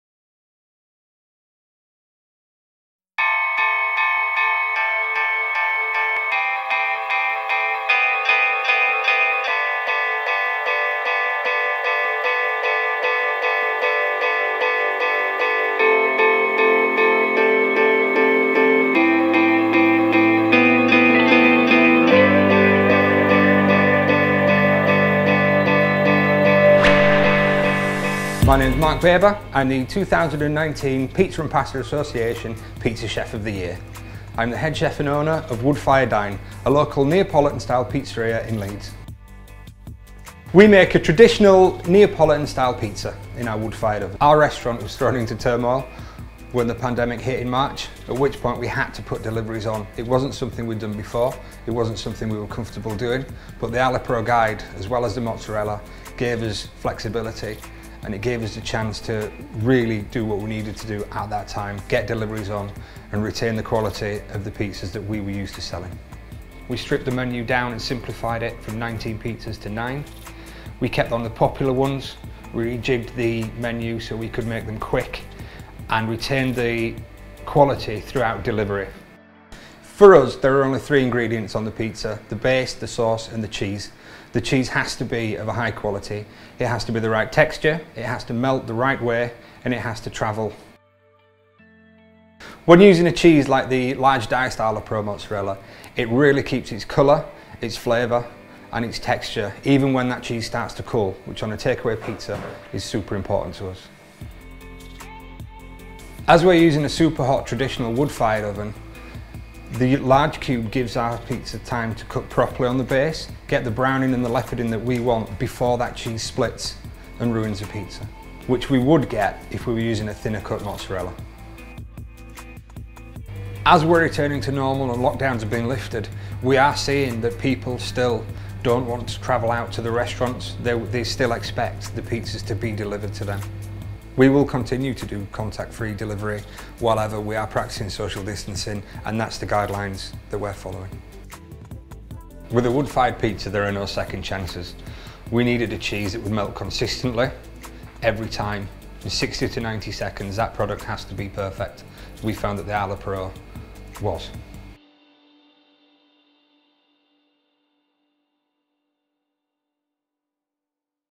Be a PRO at Pizza Delivery ft. Mark Baber at Wood Fire Dine
Watch as Mark, co-owner of Wood Fire Dine talks about the challenges faced during the coronavirus outbreak and how he had to quickly adapt his business to delivery. Supported by the Arla Pro Pizza Delivery guide Mark and his team were able to successfully deliver tasty pizza at restaurant quality.